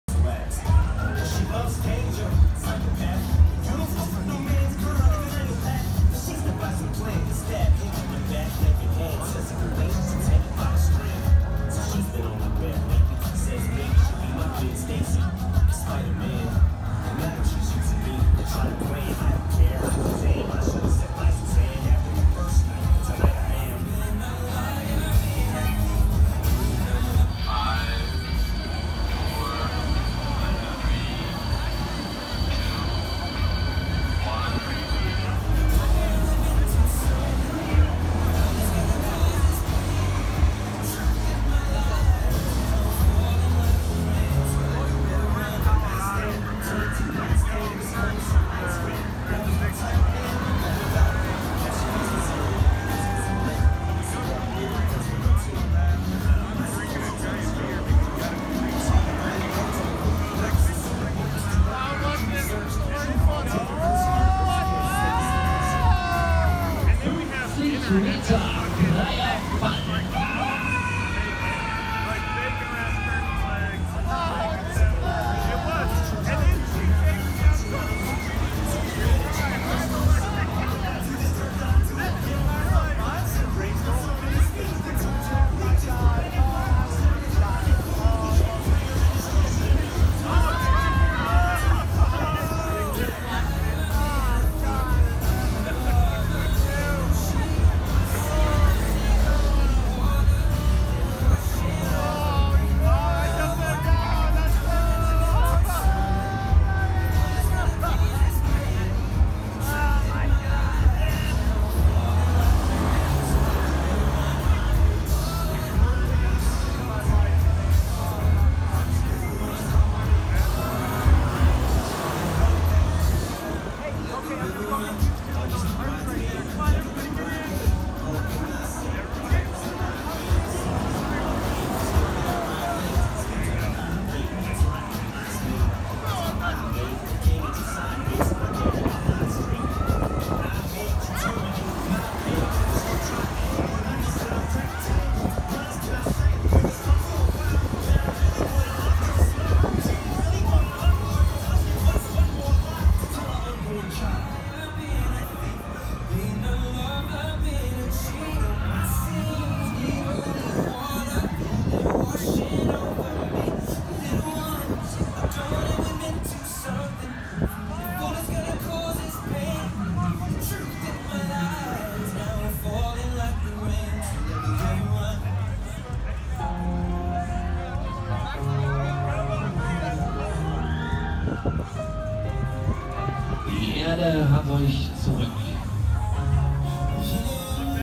Cyber Space - Kaiser (Offride) Video Frühlingsfest Nürnberg 2018
Cyber Space von Kaiser Offride Video auf dem Frühlingsfest in Nürnberg 2018. 50 Meter hoher Mondial Capriolo Cyber Space Kirmes / Volksfest Nürnberg 2018.

Fahrpreis: 6€